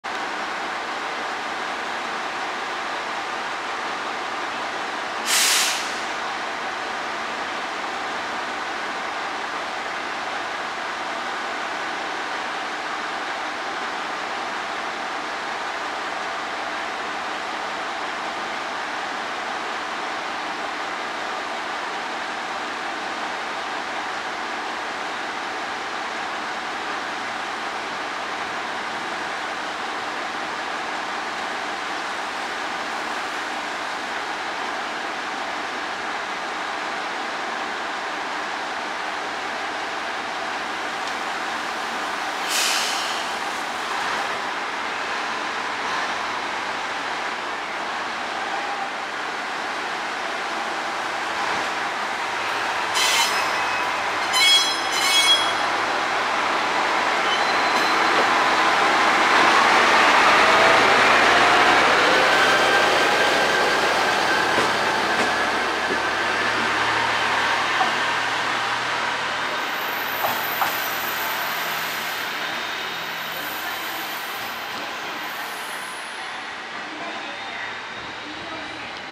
【改札口から遠い2番のりば】1500型ワンマン三本松行発車（高松駅2番のりば）
JR四国・高松駅を発車する、1500型ワンマン三本松行です。この2番のりばは改札口から多少遠いので発車間際は大変です。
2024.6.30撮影